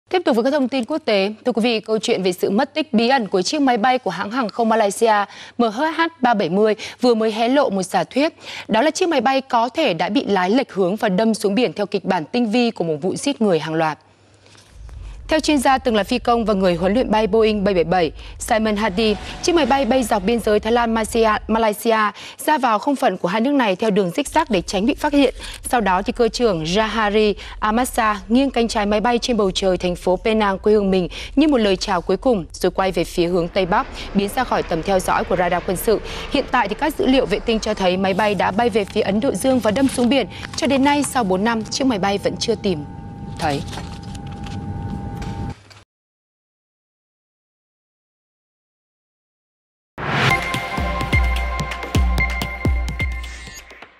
Hé lộ giả thiết về tai nạn của chiếc máy bay MH370 - Tin Tức VTV24
Câu chuyện về sự mất tích bí ẩn của chiếc máy bay hãng hàng không Malaysia MH370 vừa mới hé lộ một giả thiết: Chiếc máy bay có thể đã bị lái lệch hướng và đâm xuống biển theo kịch bản tinh vi của một vụ giết người hàng loạt.